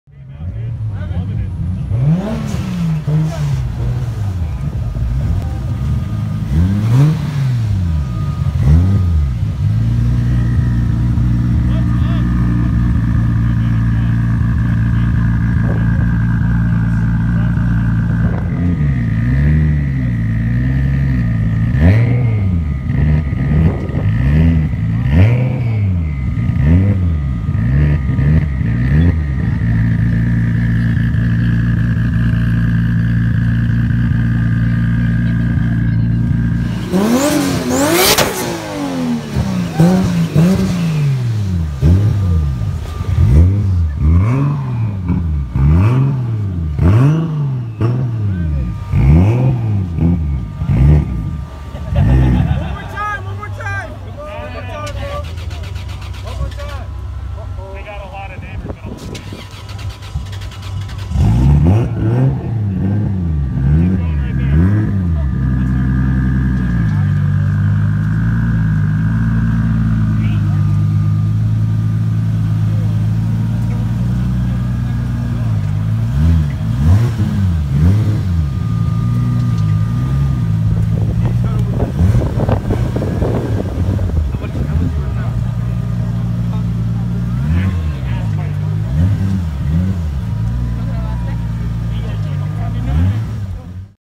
Loud MK4 Supra Pulls up to Car Event!
Loud Toyota Supra MK4 pulls up to a Cars and Coffee event in Fairfield, New Jersey.

📍Car Lofts (Fairfield, New Jersey)